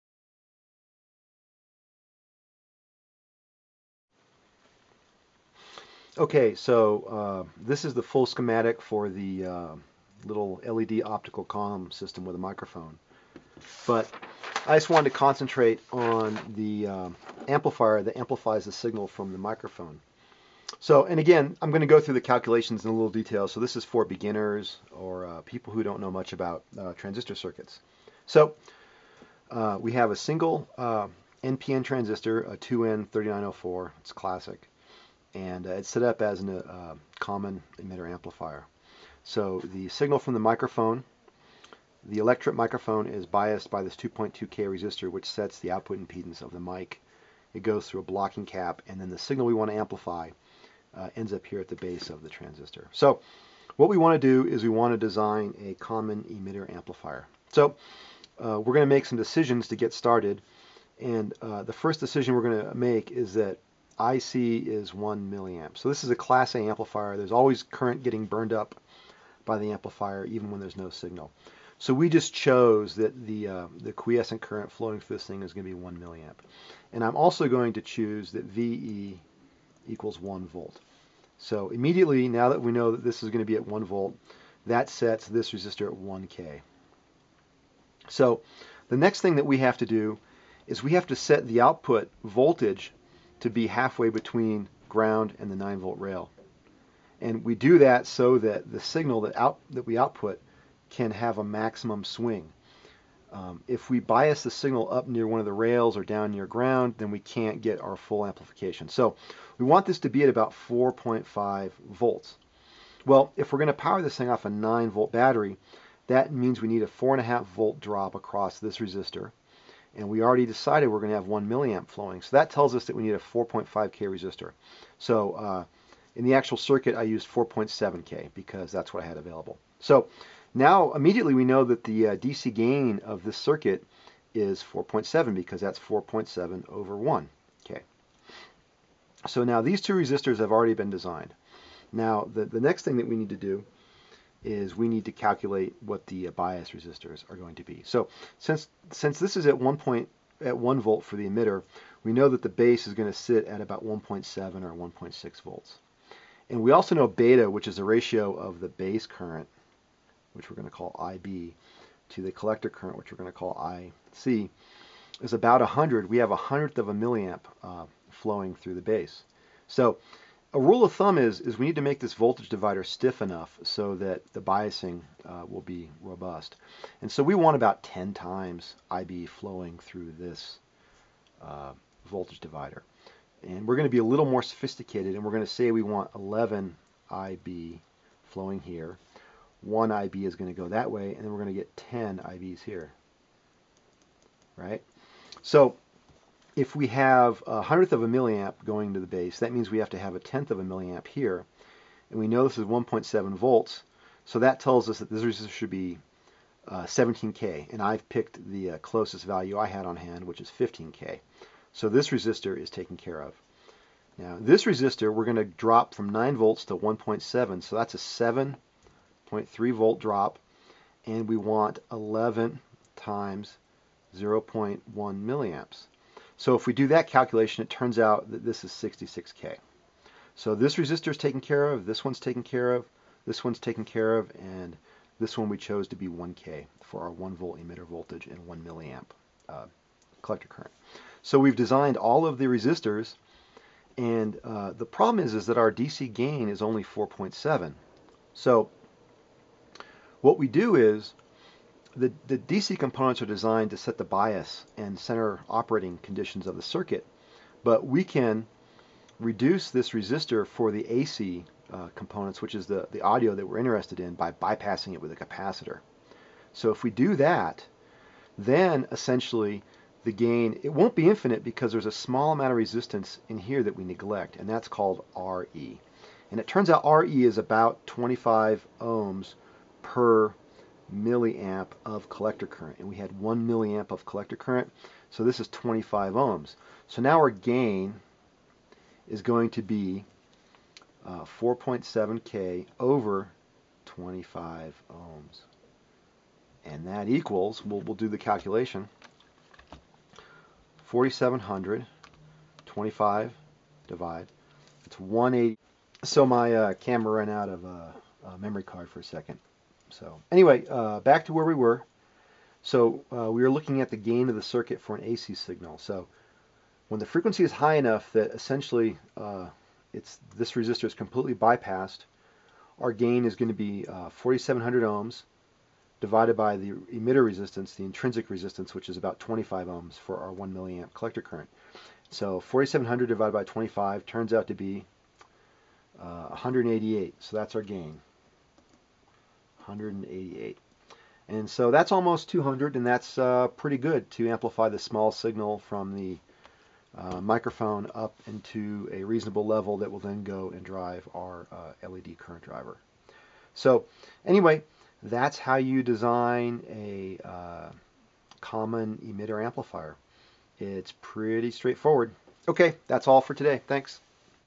Design a Common Emitter Amplifier
A walk through a simple audio common emitter amplifier design for beginners.